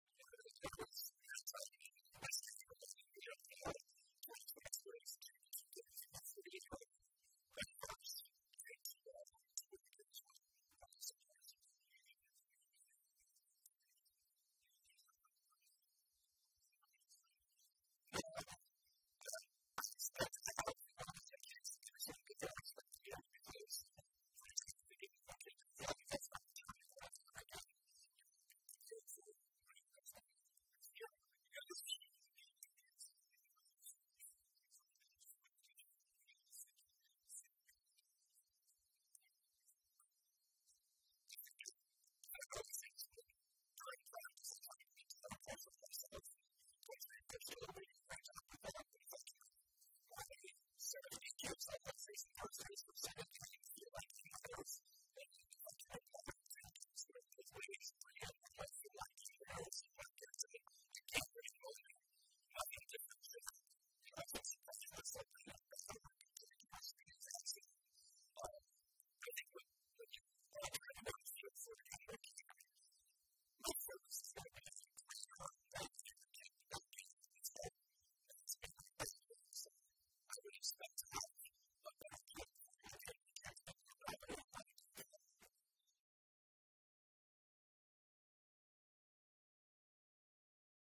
Chiefs Harrison Butker On Game-Winning Field Goal Over The Bengals
Kansas City Chiefs Harrison Butker talks about the game-winning field goal Sunday night, Sept. 15, 2024,  against the Bengals.